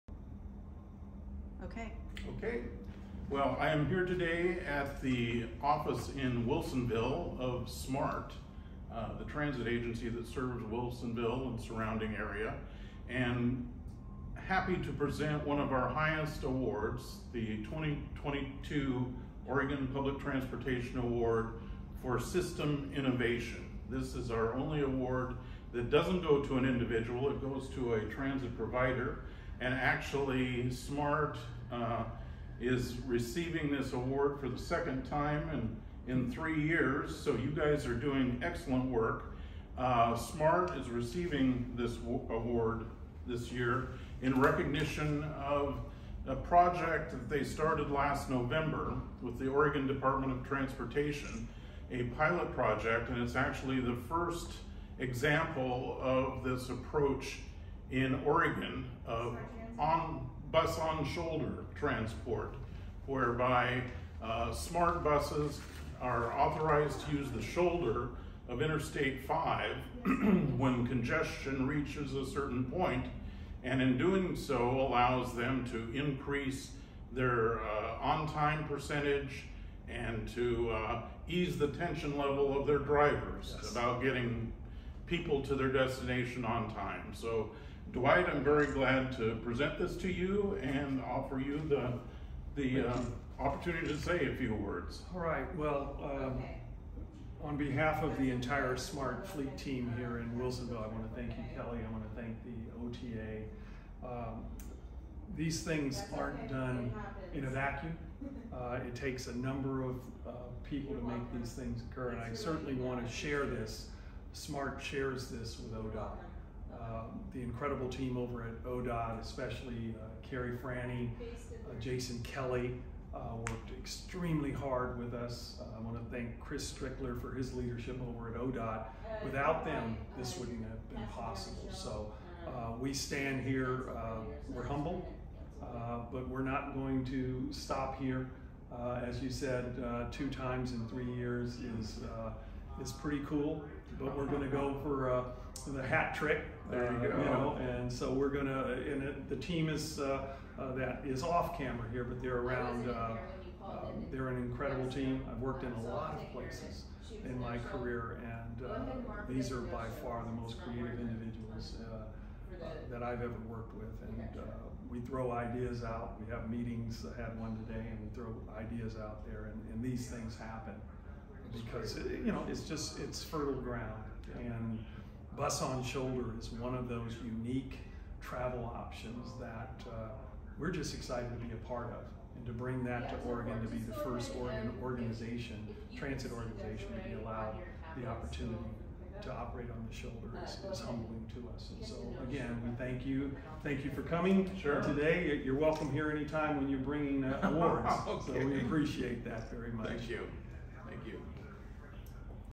Oregon Transit Association System Innovation Award
Watch as SMART transit of Wilsonville receives OTA's System Innovation Award for their Bus on Shoulder pilot. Congrats to everyone involved in this effort!